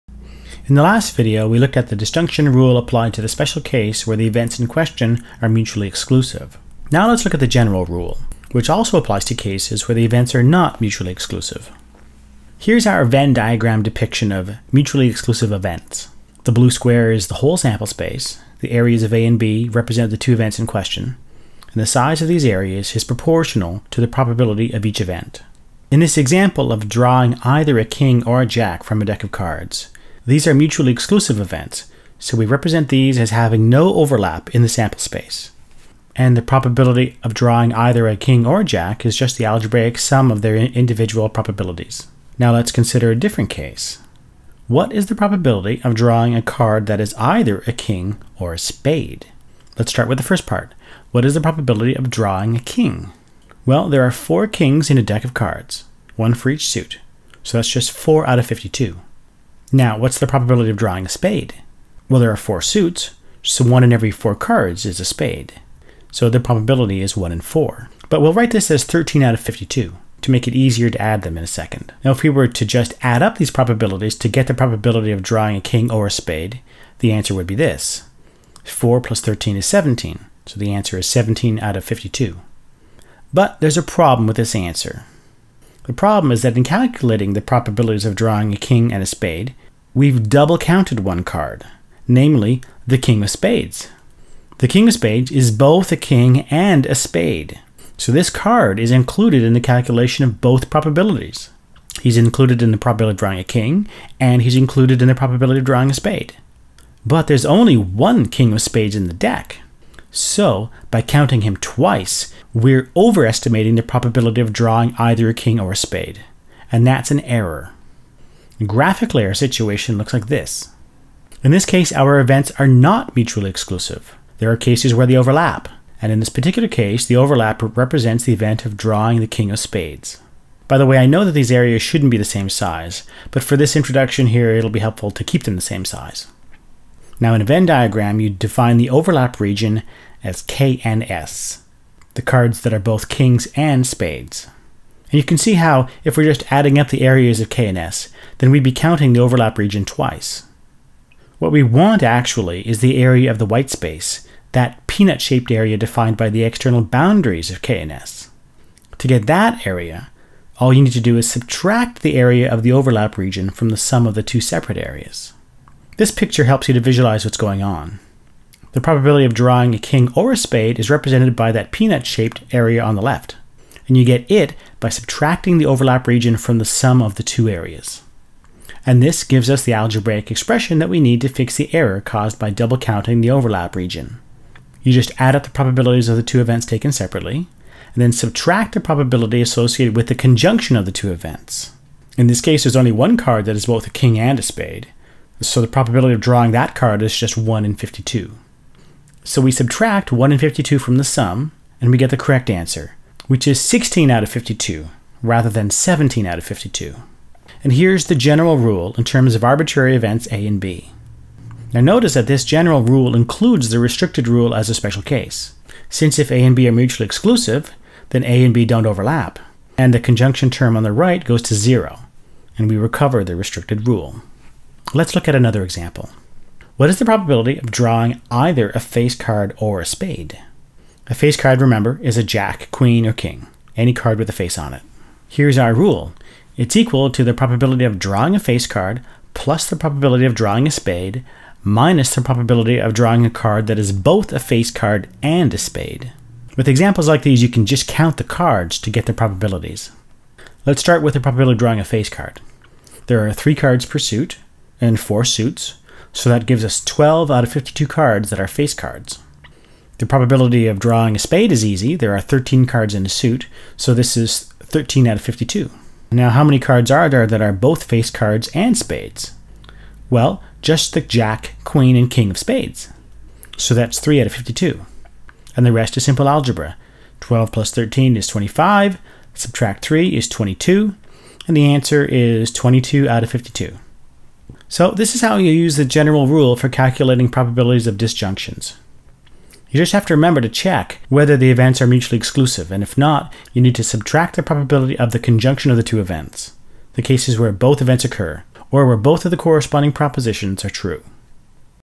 Probability: The General Disjunction Rule
This video shows how to calculate the probability of a disjunction of events (either A or B), even when the events are NOT mutually exclusive.